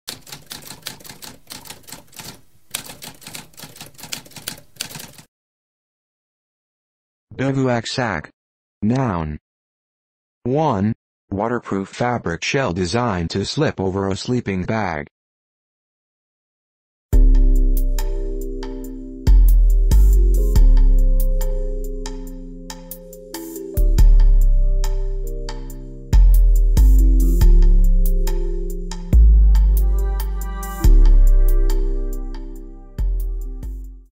What does bivouac sack mean?
A spoken definition of bivouac sack.

Intro Sound:
Typewriter - Tamskp